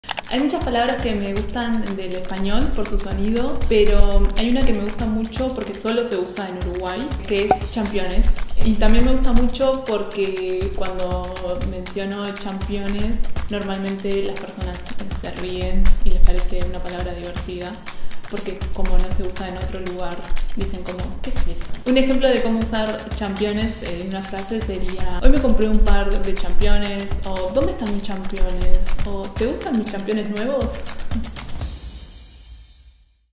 La palabra es... ¡Championes!
La palabra es... ¡championes! ¿Por qué crees que nuestra profesora Alejandra la ha elegido? ¡Descubre en el vídeo!

The word is... championes! Why do you think our teacher Alejandra has chosen it? Find out in the video!